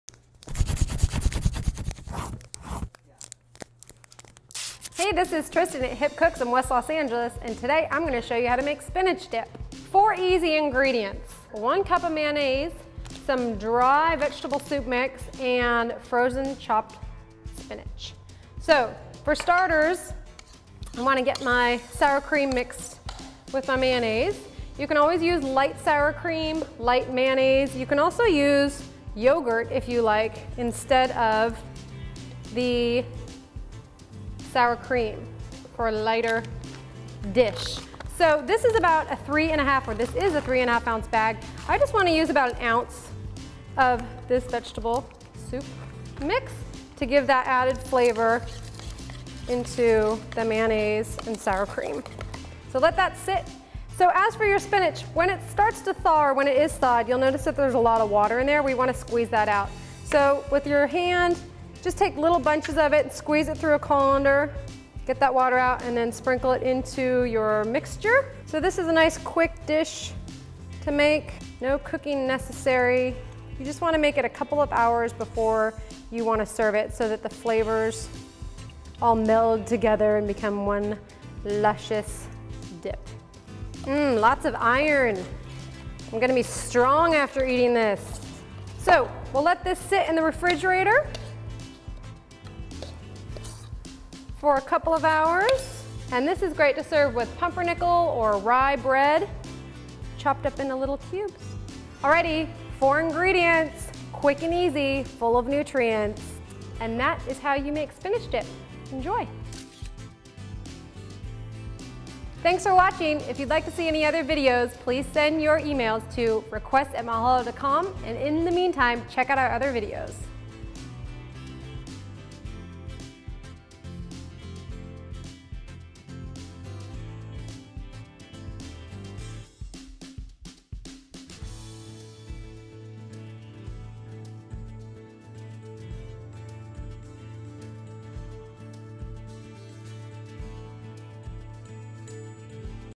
How to Make Spinach Dip
Learn how to make spinach dip with the help of Mahalo and Tristan! Enjoy!